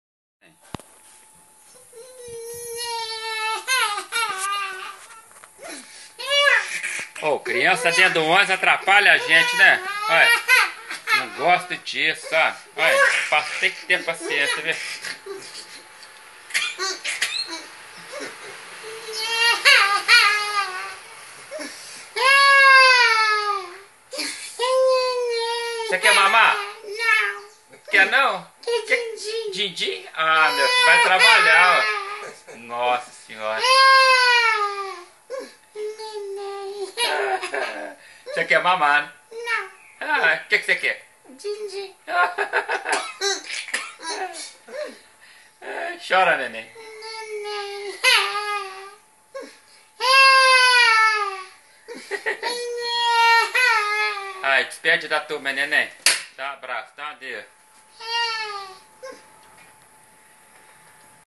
MOTORISTA SE IRRITA COM BEBÊ CHORÃO
Motorista fica irritado com bebê que chora dentro do ônibus
Esse Brasil tem muitos artistas escondidos, kkkkkk!